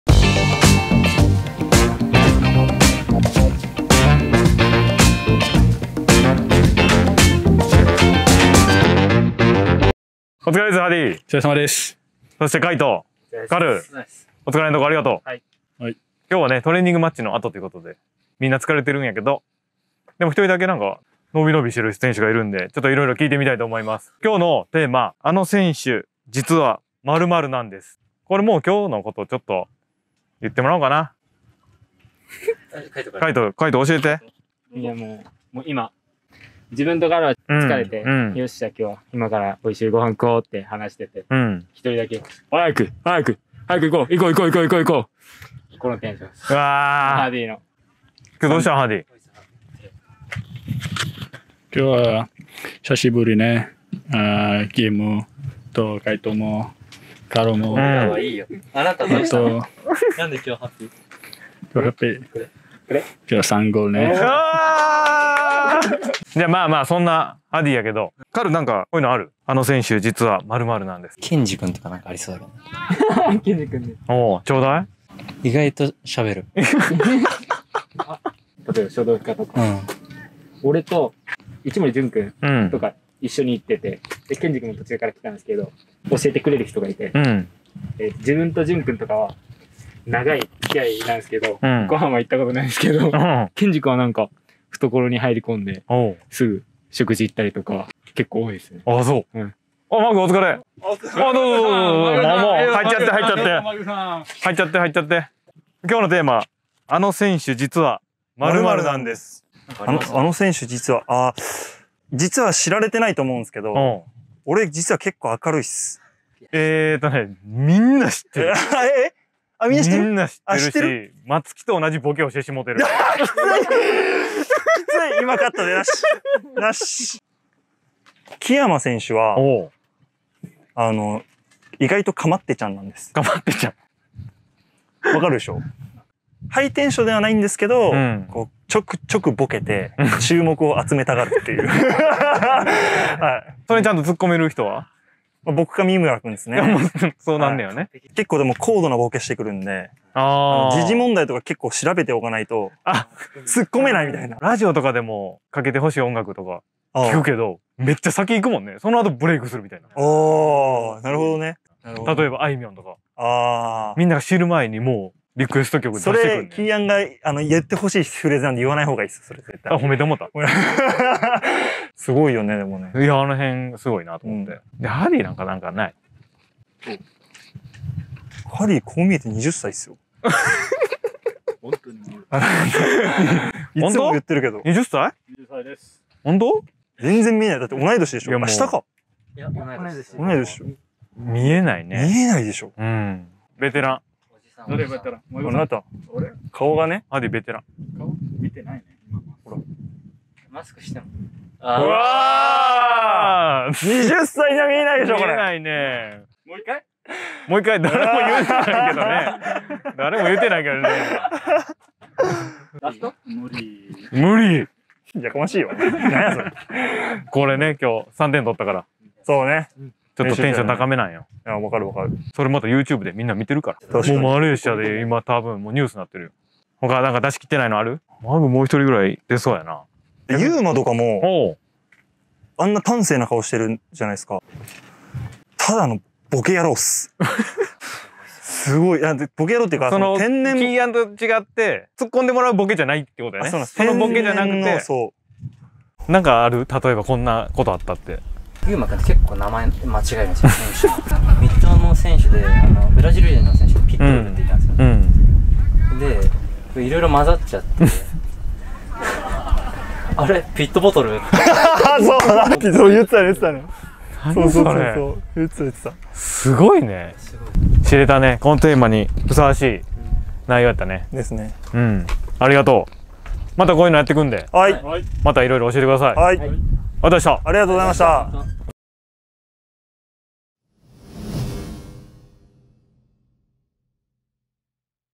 ＃003-3【月～金更新】政田 INSIDE INTERVIEW
ファジアーノ岡山の練習拠点『政田（まさだ）サッカー場』でのインタビュー企画！

広報部の瀬島（せじま）が、毎週違ったテーマについて、もはや日常会話？雑談？のような話を繰り広げます！

気心の知れたフロントスタッフによって引き出される、試合中には見ることのできない、選手の普段の表情をお楽しみください！

＼＼今週のテーマ『あの選手、実は〇〇なんです！』／／
毎日顔を合わせる選手同士だからこそ知っている秘密！
知られざる意外な一面を暴きます！

トレーニングの様子を伝える『政田 INSIDE TRAINING』など公式ブログはこちら↓

※制作、スケジュールなどの都合により、配信ができない場合がございます。予めご了承ください。